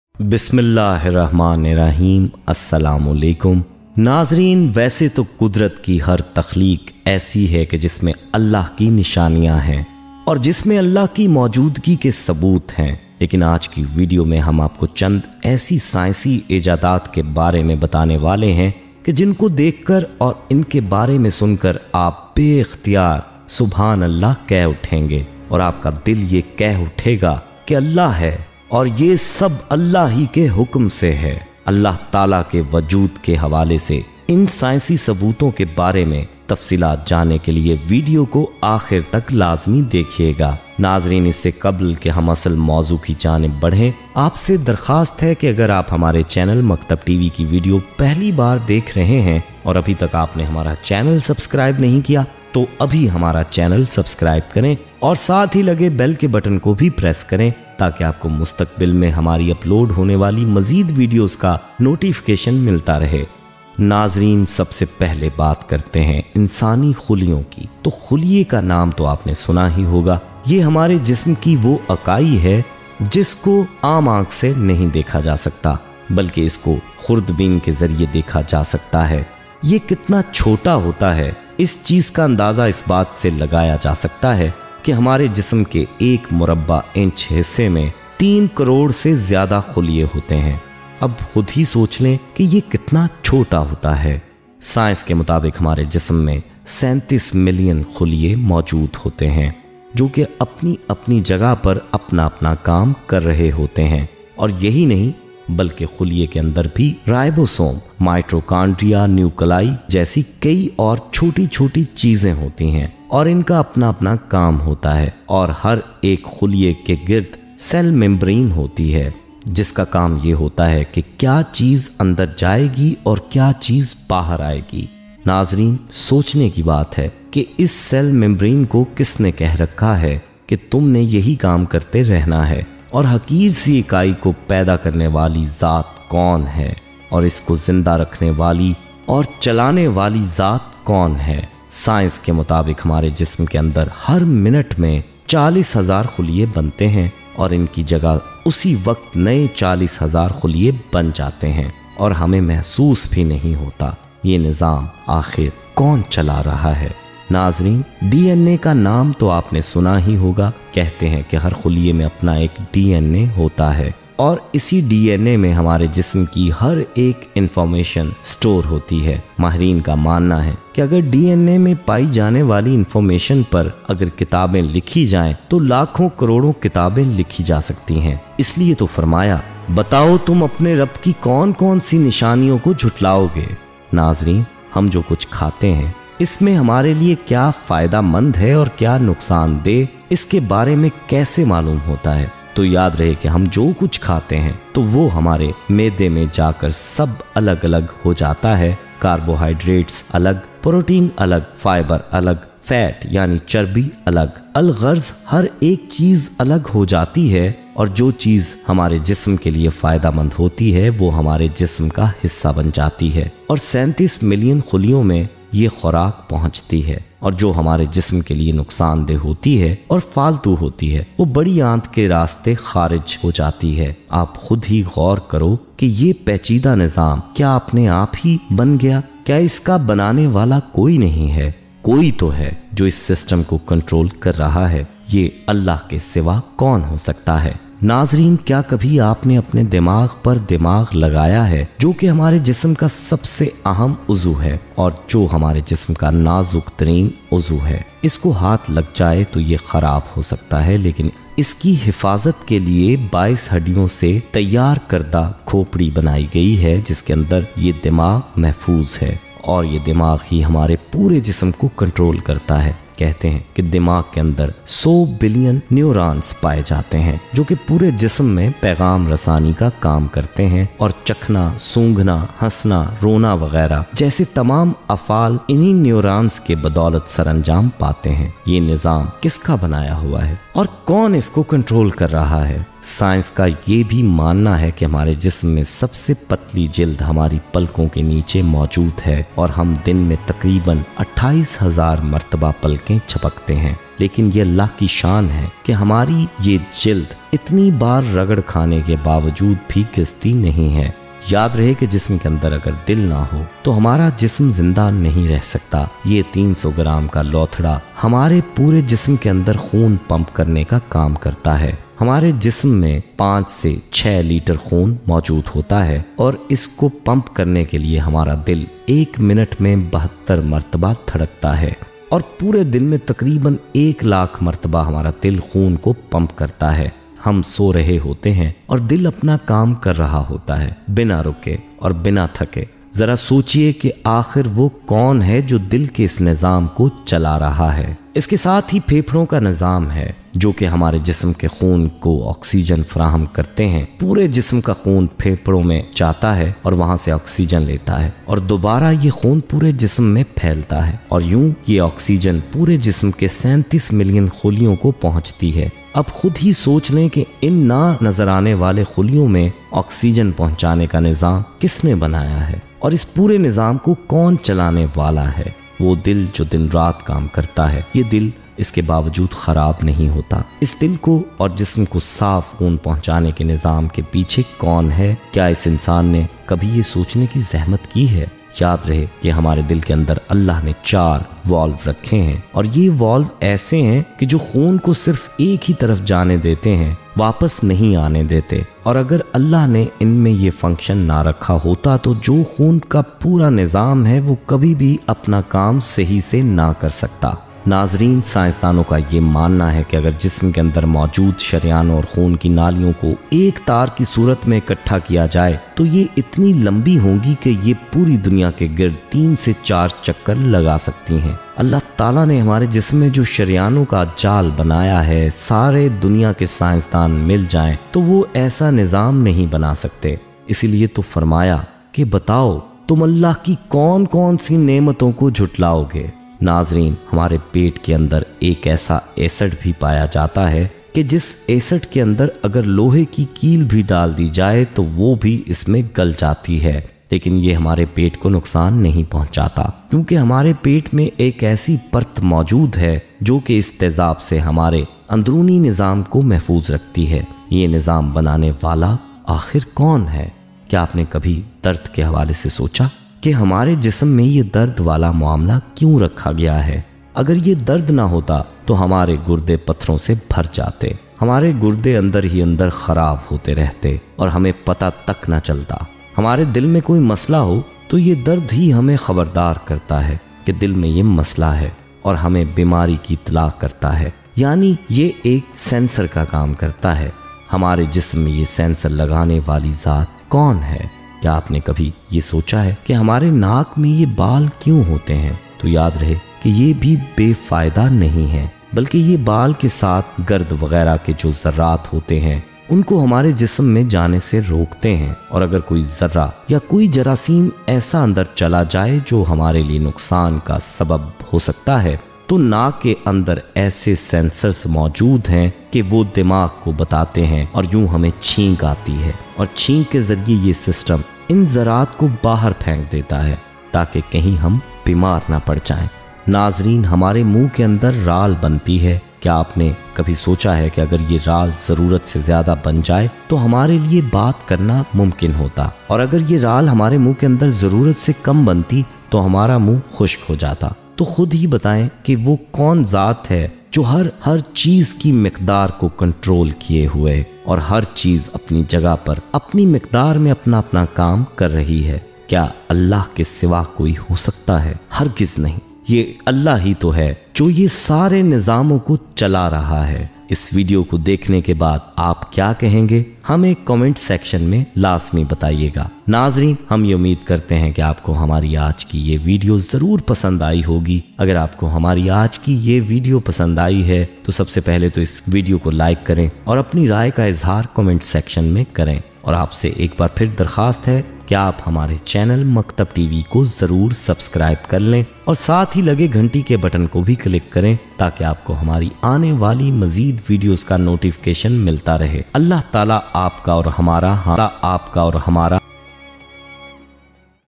Allah ki nishaniyan - Science bhi dekh kar hiraan - Maktab TV
Aaj ki is video mein hum apko Allah ki Nishaniyann dikhaein gy jo ke insani jism mein poshida hein is video ko pura dekhein aur aagay bhi share krein, JazakAllah!